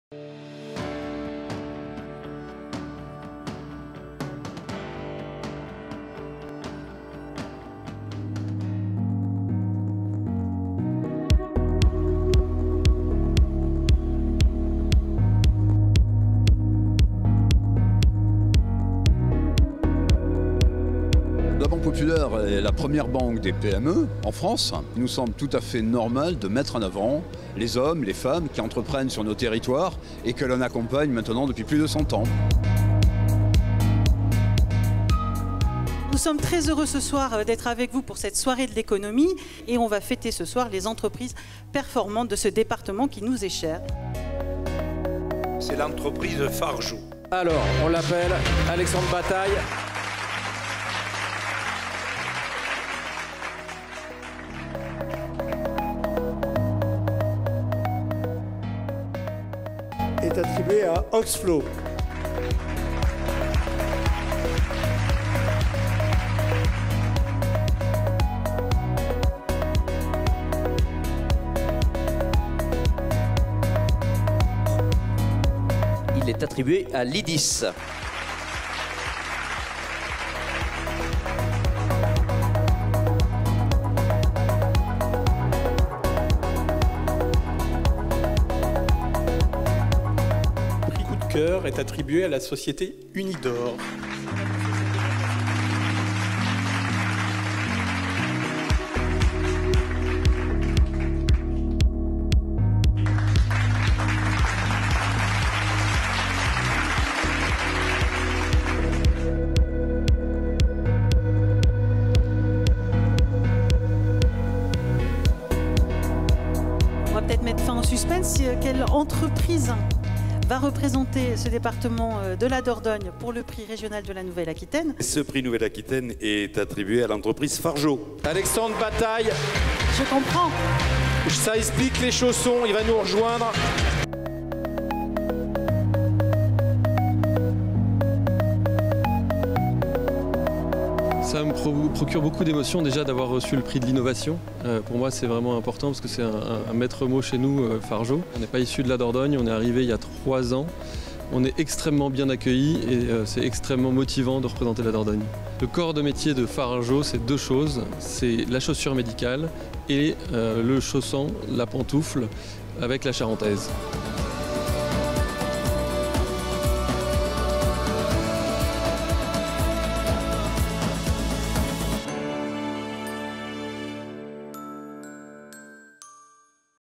Prix de l’Éco Néo-Aquitains, Dordogne
Cinq entreprises lauréates et le grand gagnant de la soirée : Fargeot qui représentera la Dordogne lors de la finale régionale.